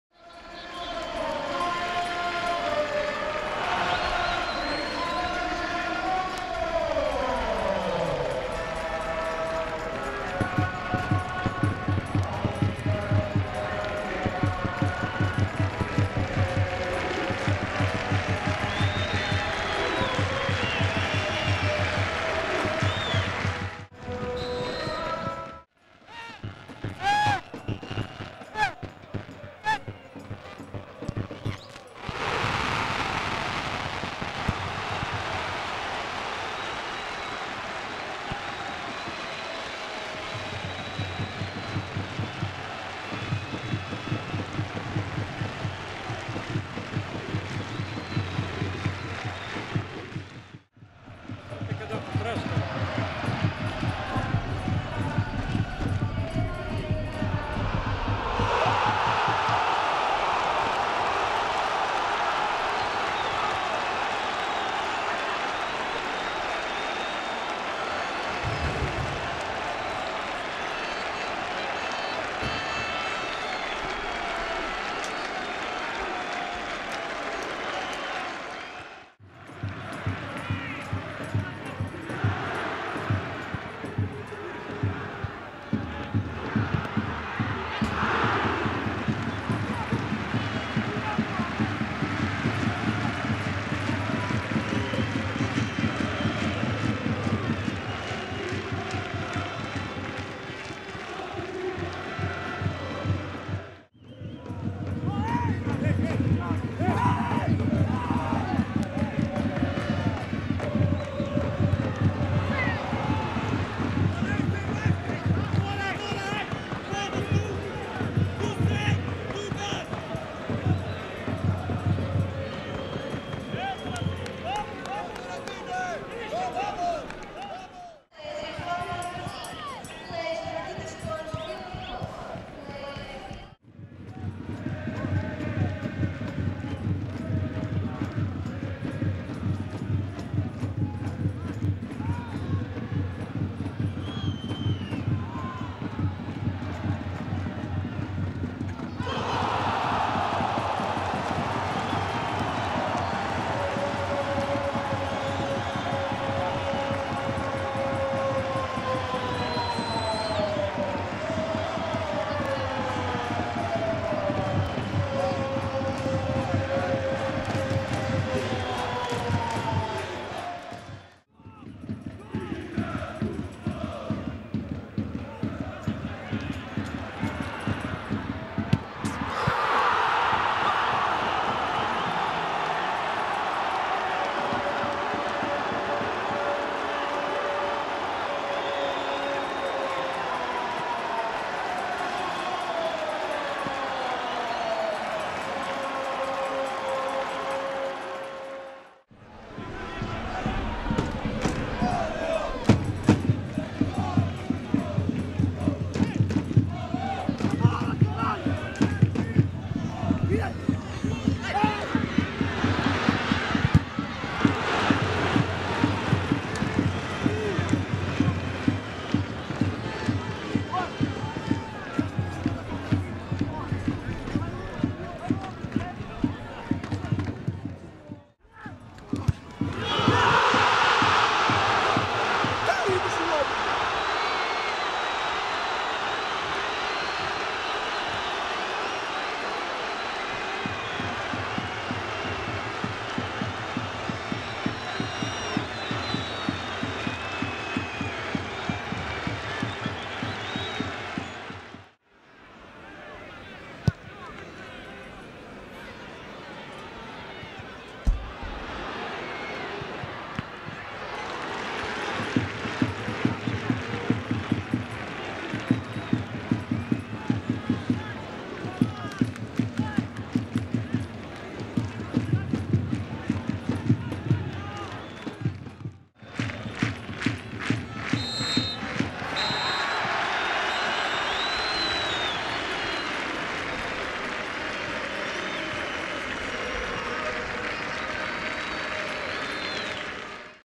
Rio Ave 2-3 Sporting (5ª Jornada - Época 11/12)
O Sporting teve uma «entrada de leão» com dois golos nos minutos iniciais, mas permitiu a recuperação vilacondense antes de Onyewu «subir ao segundo andar» (73 m) para garantir os três pontos.